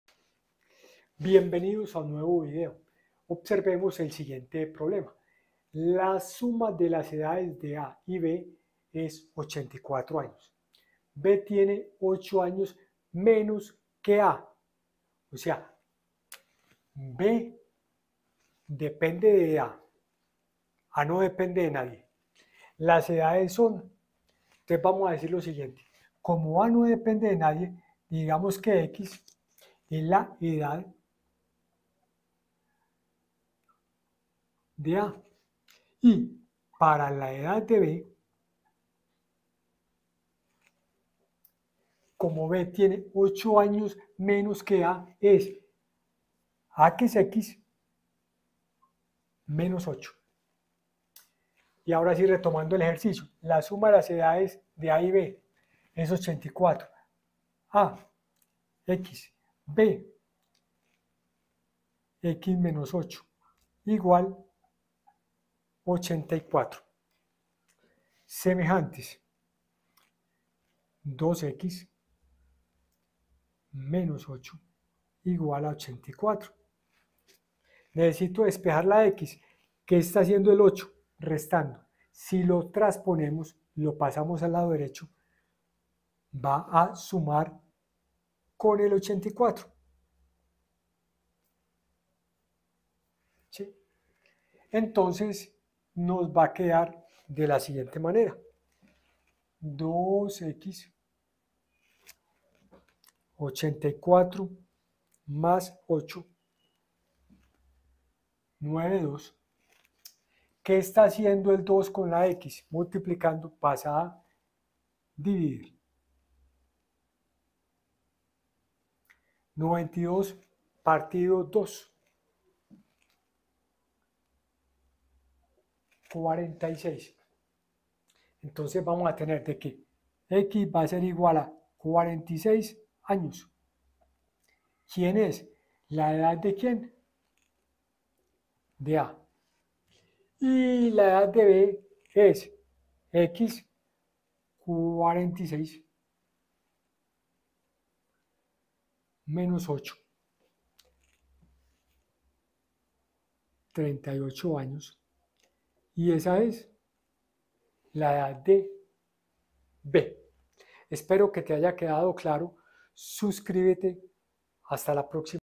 Problema de aplicación. Ecuación 1x1. Video 13.
profenini te explica como plantear y resolver una ecuación 1x1.